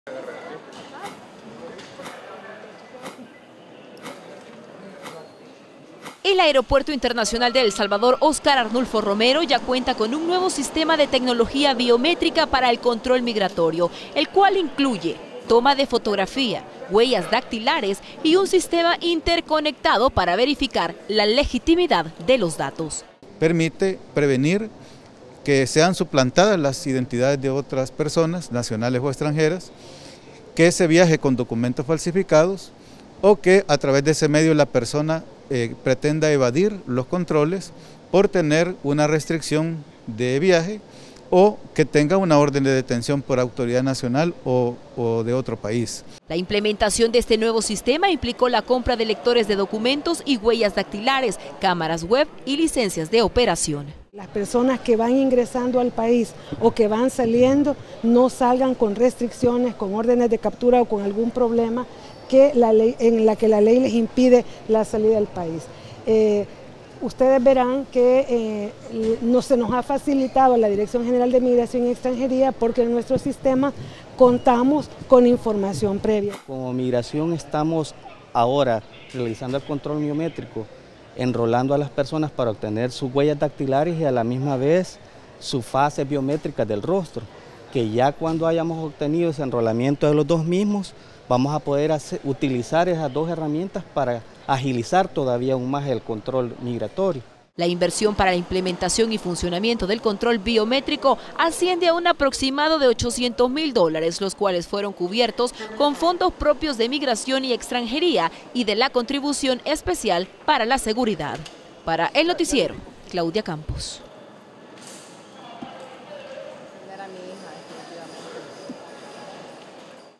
Implementan sistema de biometría en el aeropuerto San Óscar Arnulfo Romero
Hoy autoridades de seguridad y migración presentaron el nuevo sistema de tecnología biométrica para el control migratorio en el aeropuerto internacional San Óscar Arnulfo Romero.